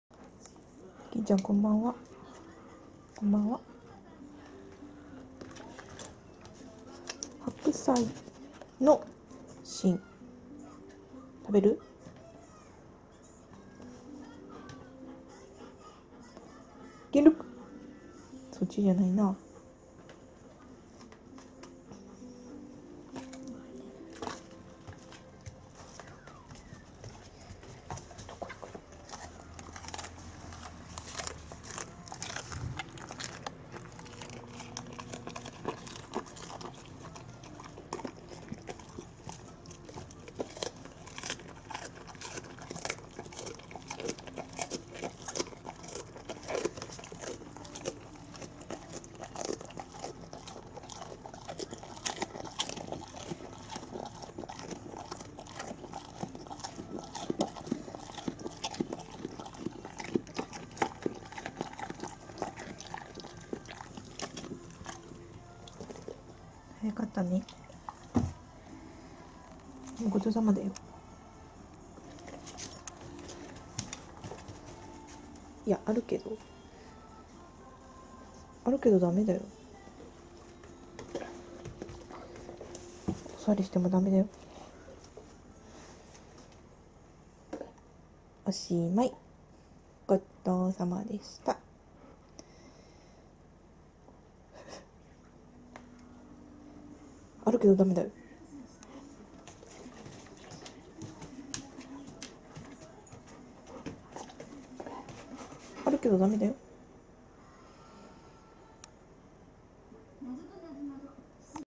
【ギンのASMR】白菜の芯は意外と甘くてクセになると悟ったバーニーズマウンテンドッグのギン
冷蔵庫で眠ってた白菜の芯を言い音させながら食べるギンさんの様子♪

ちなみに、残りの白菜は我が家の夜のスープになりました☆